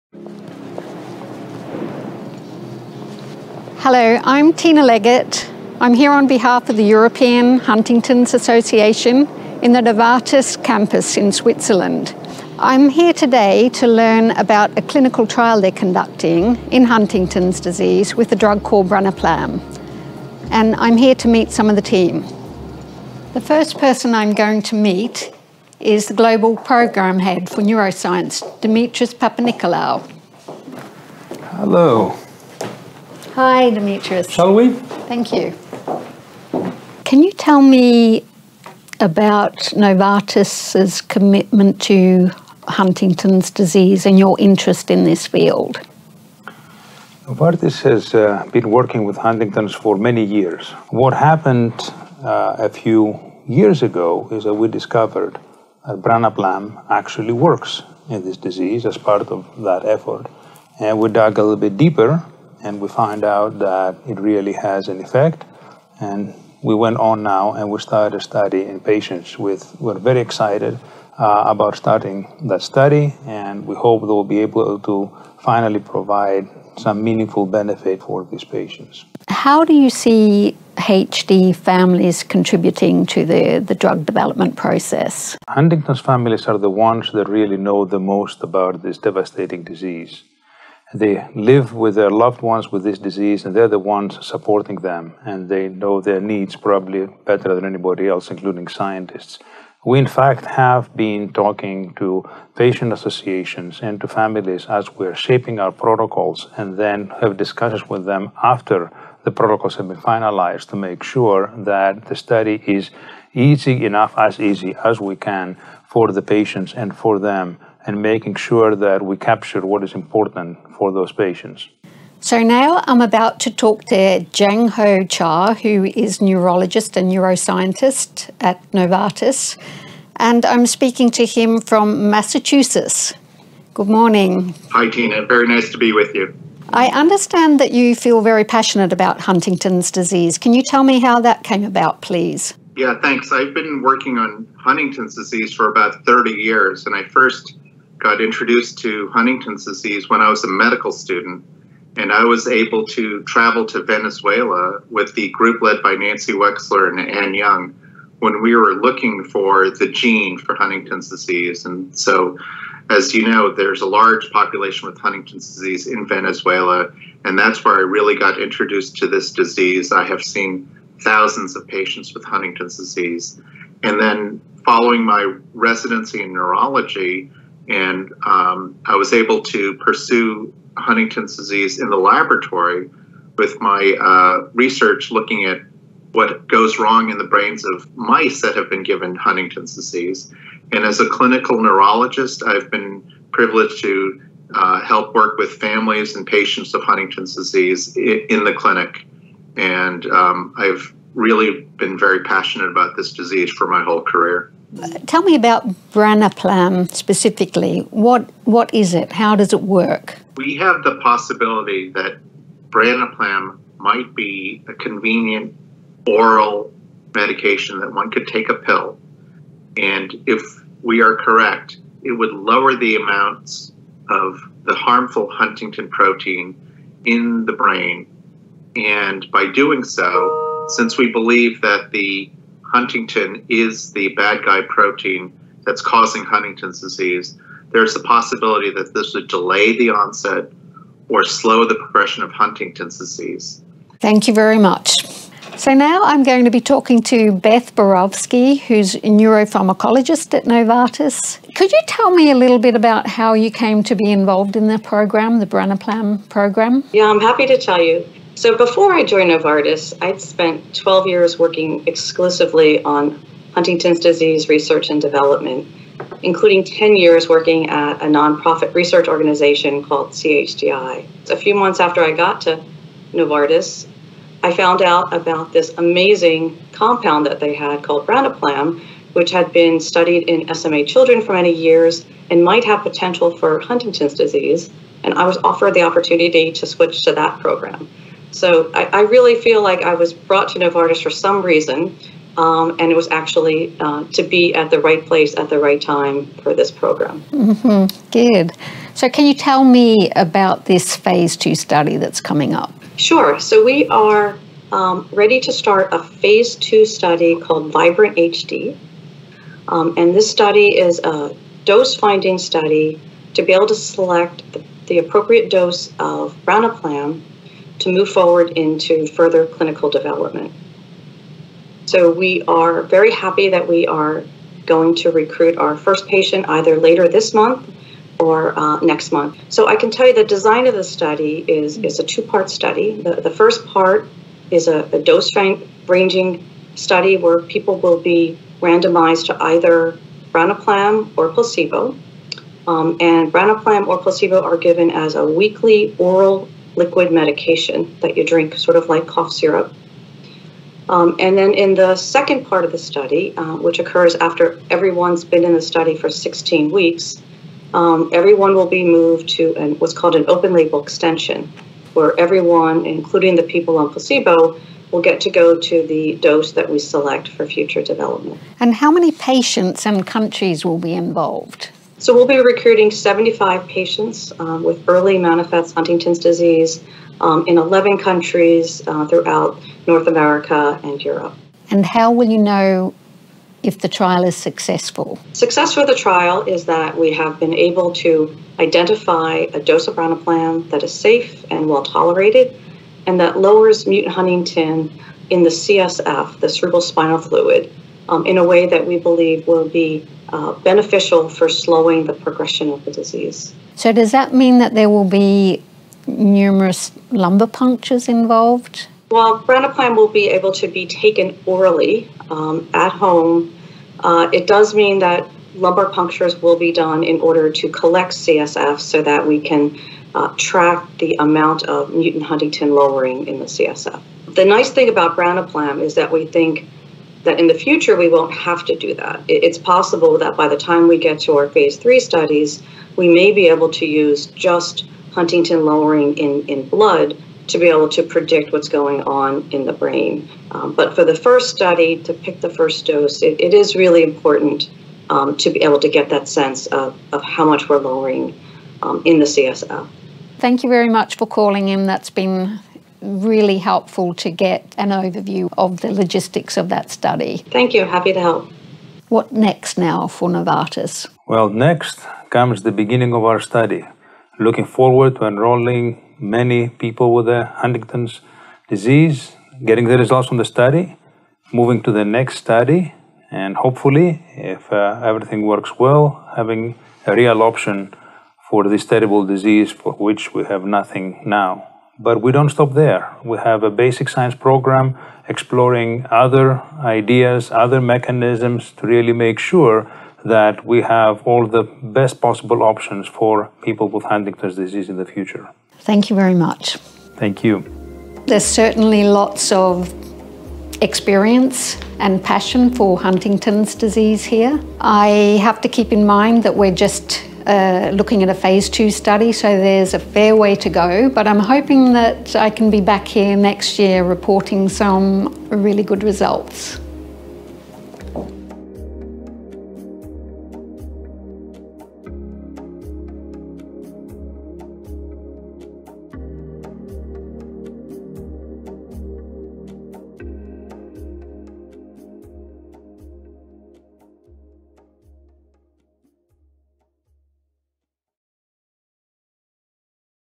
Vibrant HD - Novartis clinical trial for Huntington's Disease
Tina Leggett, on behalf of the European Huntington Association, travelled to the Novartis Campus in Switzerland. She interviewed Dimitri Papanicolaou, Jang Ho-Cha and Beth Borowsky, to learn more about Vibrant HD, a Phase 2 clinical trial that Novartis is conducting where they test a drug named Branaplam.